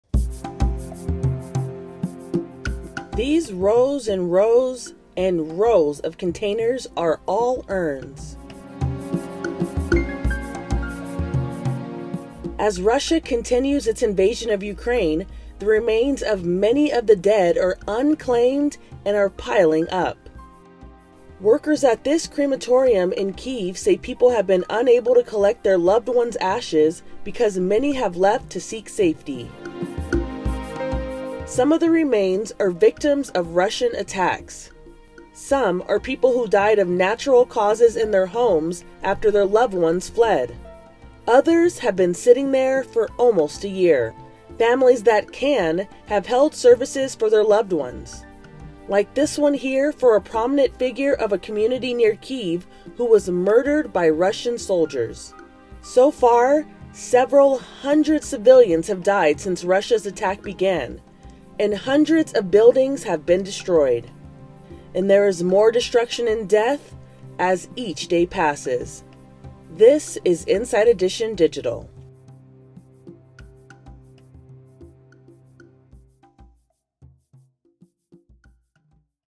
These Urns in Kyiv Are All Unclaimed
As Russia continues its invasion of Ukraine, the remains of many of the dead are unclaimed and are piling up. Workers at this crematorium in Kyiv say people have been unable to collect their loved ones’ urns because many have left to seek safety. Most of the remains are either victims of Russian attacks or elderly who have died of natural causes in their homes after their loved ones fled. Inside Edition Digital has more.